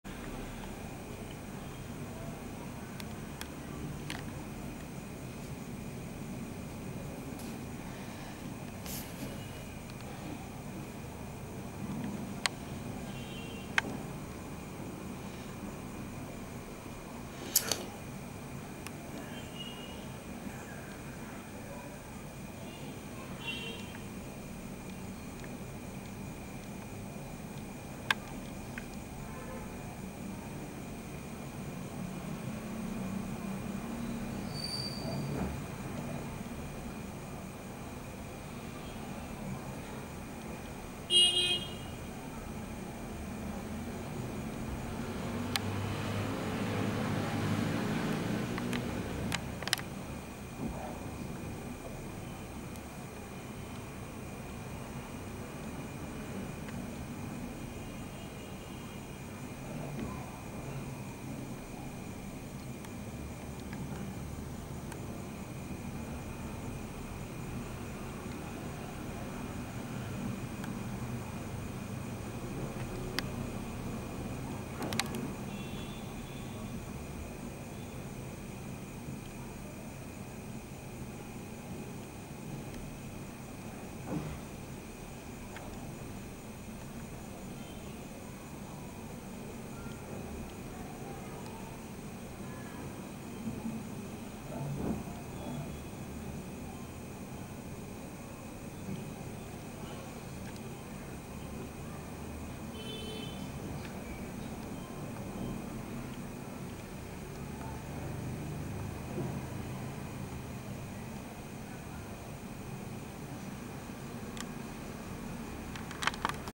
Genus Trabala Moth
Shrikant Kelkar's video clip taken at Kalwa, Maharashtra, India in October 2019

Family : Lasiocampidae ; Subfamily : Lasiocampinae